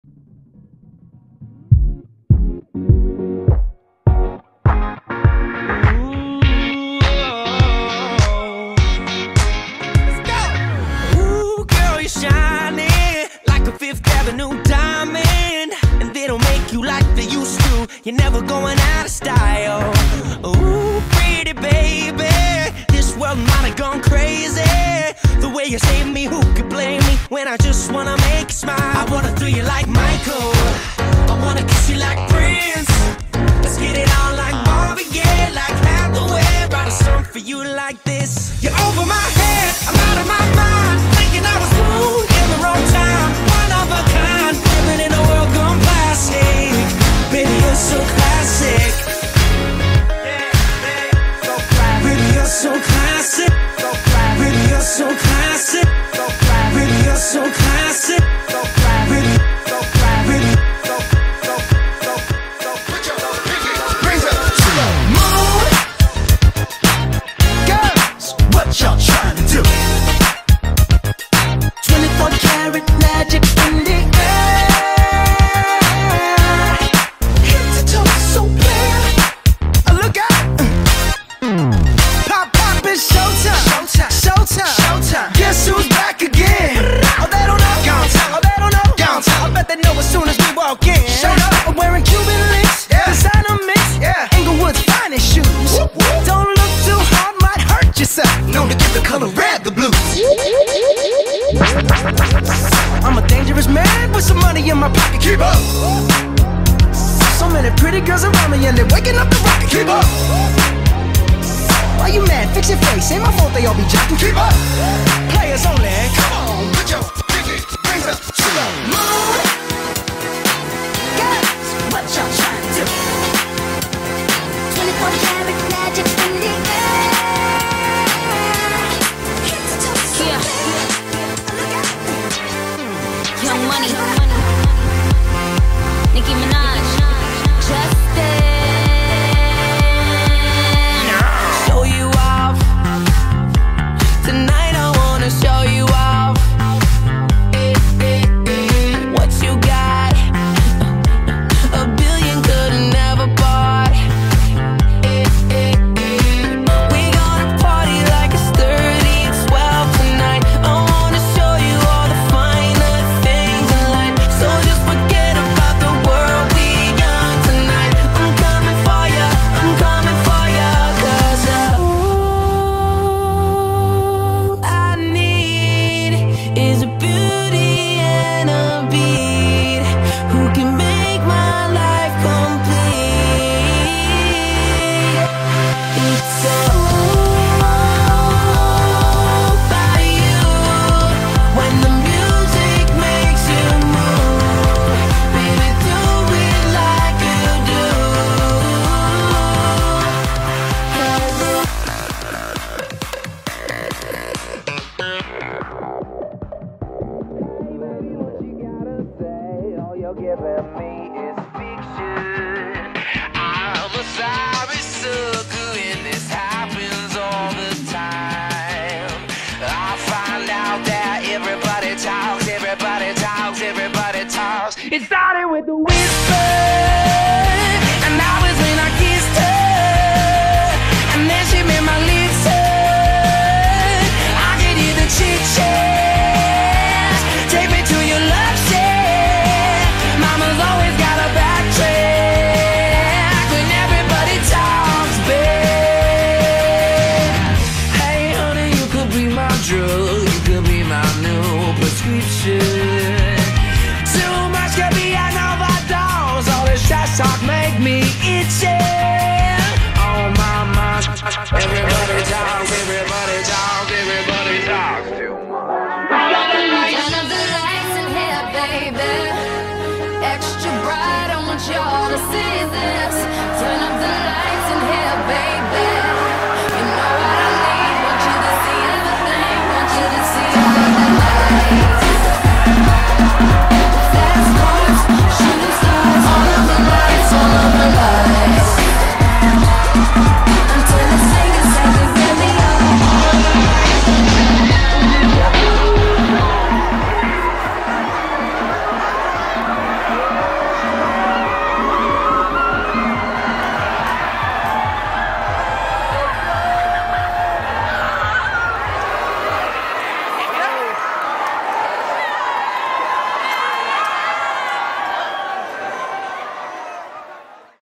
Mars Area High School - Lip Dub 2024
Mars Area High School Student Council has produced a school-wide Lip Dub performance, highlighting the school’s numerous activities, clubs and sports teams.

Earlier this year, several Student Council members brought up the idea of bringing the Lip Dub back to Mars Area High School, says Meghan Boland, Student Council advisor, noting that the high school last produced a Lip Dub in 2013 as part of a now defunct regional contest.